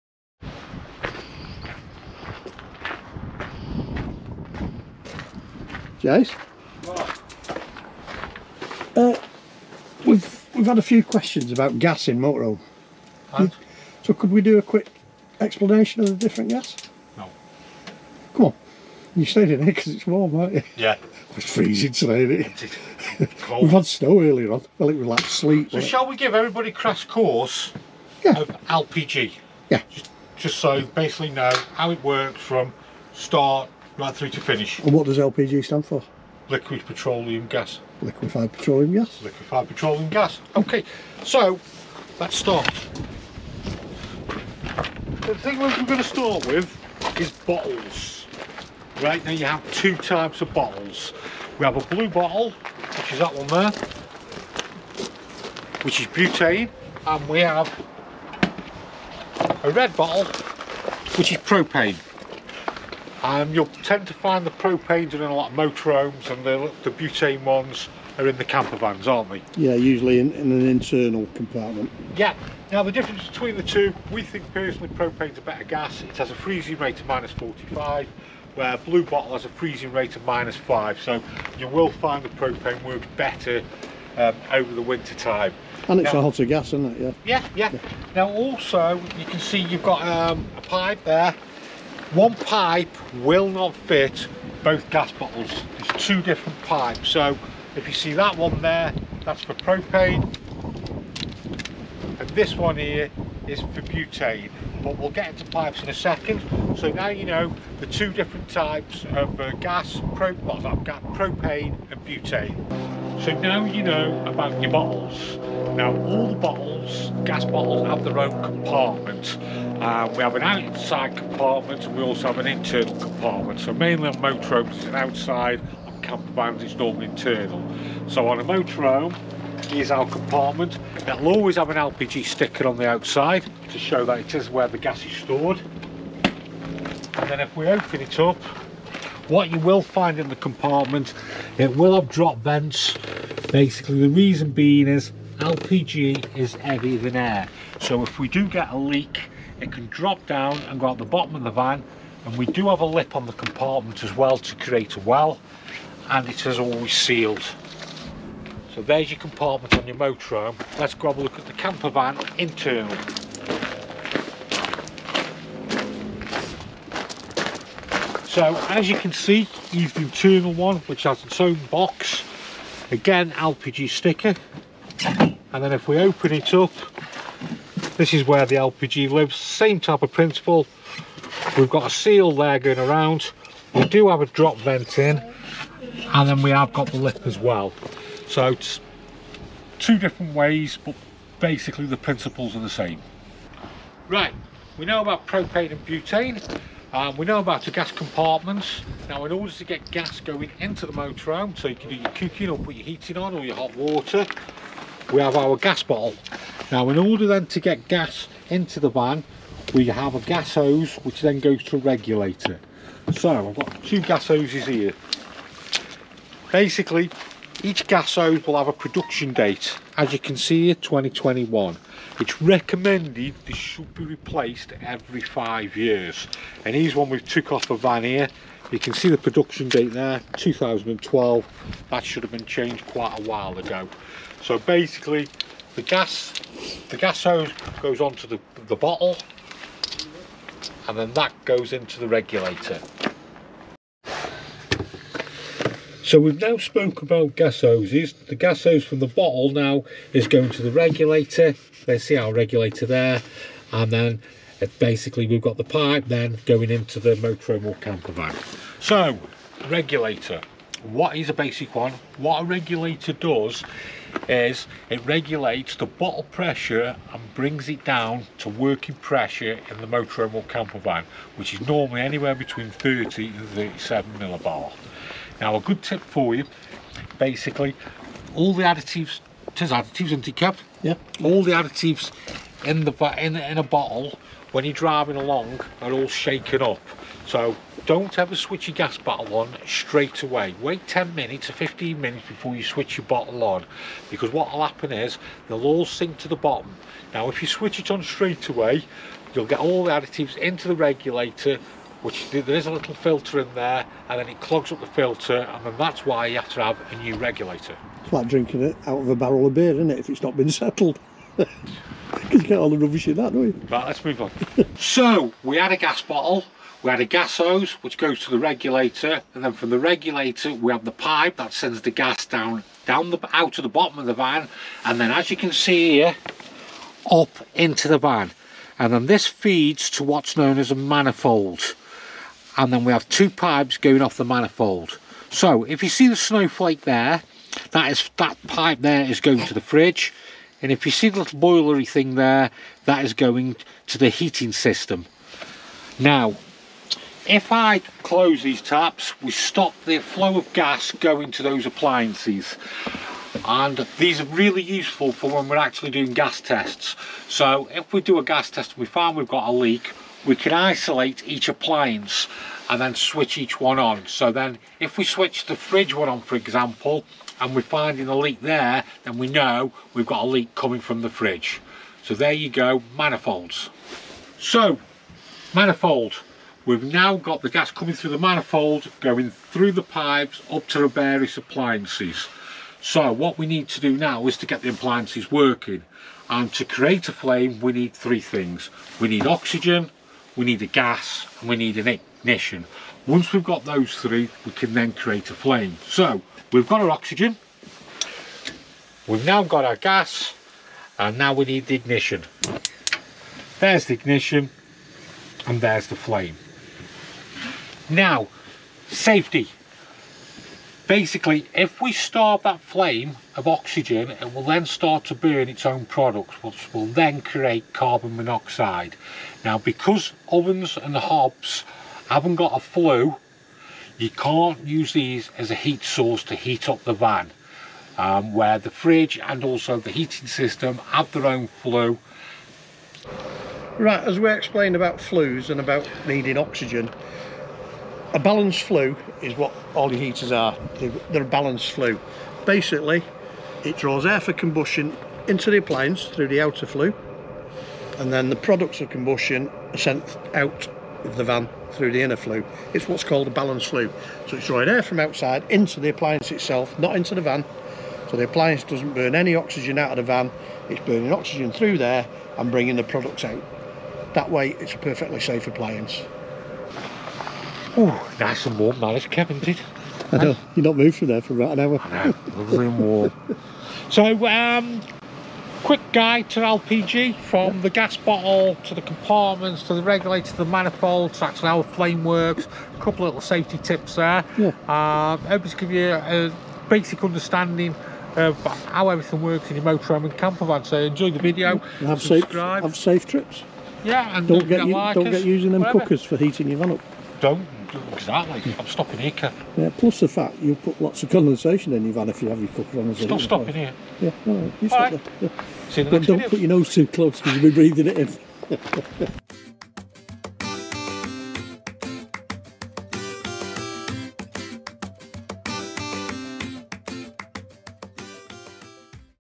CAMPERVAN GAS ! How It Works In Your Motorhome
How your Motorhome and Campervan LPG Gas,Propane,Butane Works from the Bottle to your aplliances
In this video we cover LPG Gas on your Motorhome and Campervan giving you a basic understanding of Propane and Butane and do's and don'ts.

Looking to Sell Your Motorhome - We Buy all types of Motorhomes and Campervans.

TikTok.      The Motorhome Man
Instagram The Motorhome Man